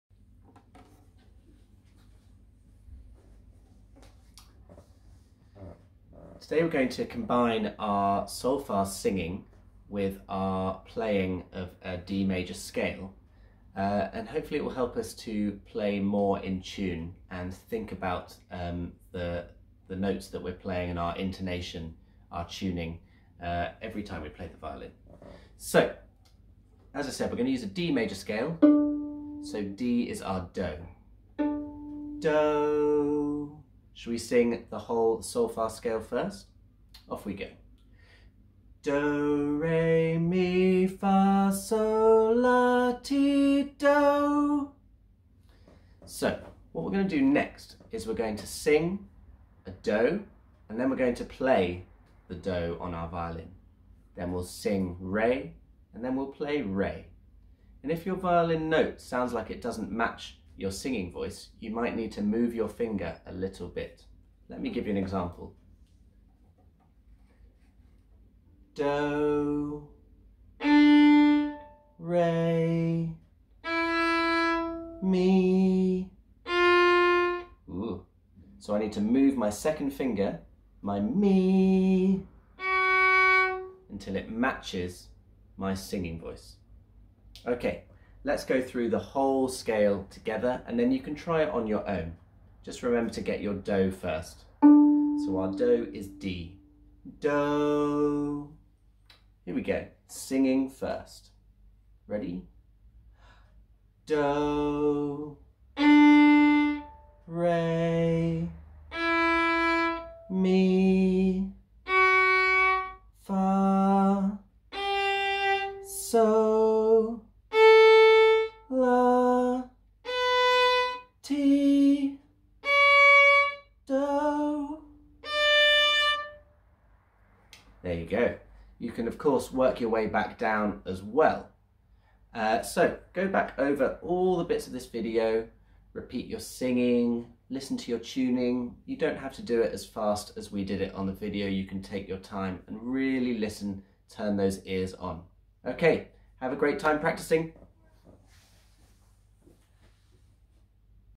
Sing + Scale : D Major (Solfa + Violin)
How to use your singing voice & solfa to improve your tuning on a D Major scale.